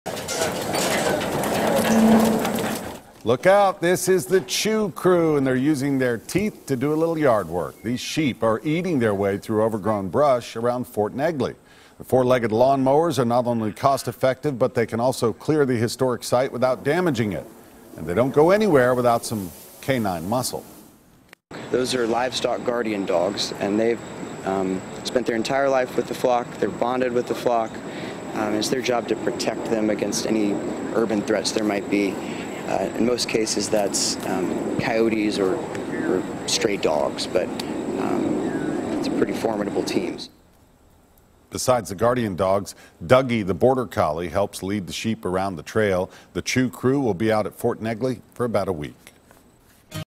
Sheep Maintain Fort Negley Property
A heard of sheep have taken on a new role of landscapers at Fort Negley.